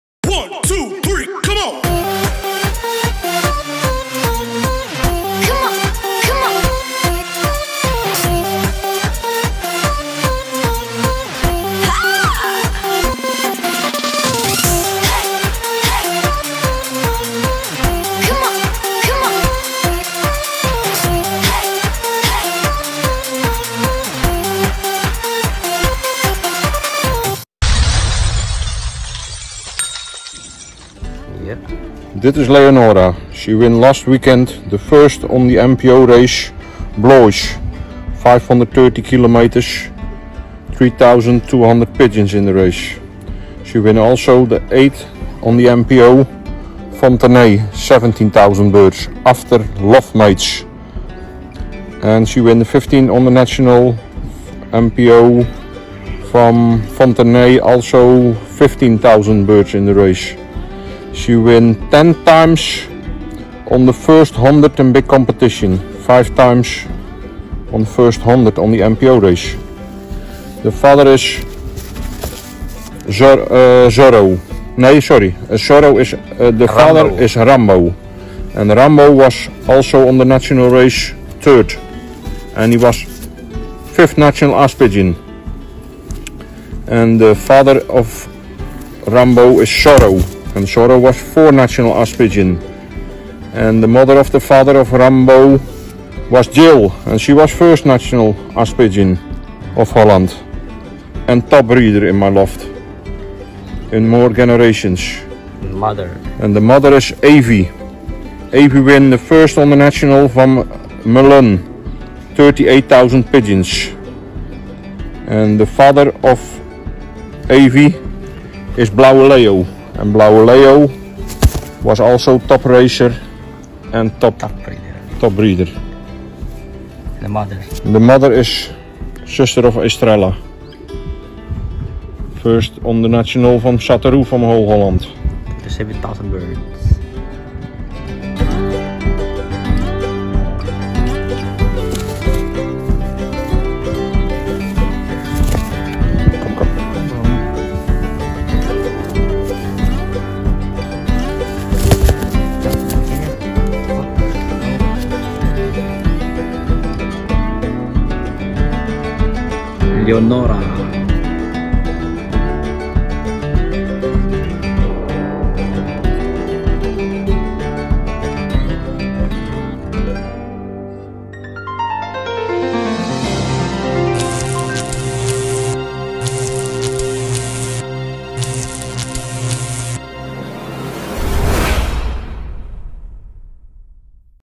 Peter Van De Merwe "LEONORA" 1st NPO BLOIS 528 km against 3,211 Birds
"LEONORA"
1st NPO BLOIS 528km
Against 3,211 Birds

daughter of
RAMBO x EVY
both parents are also winners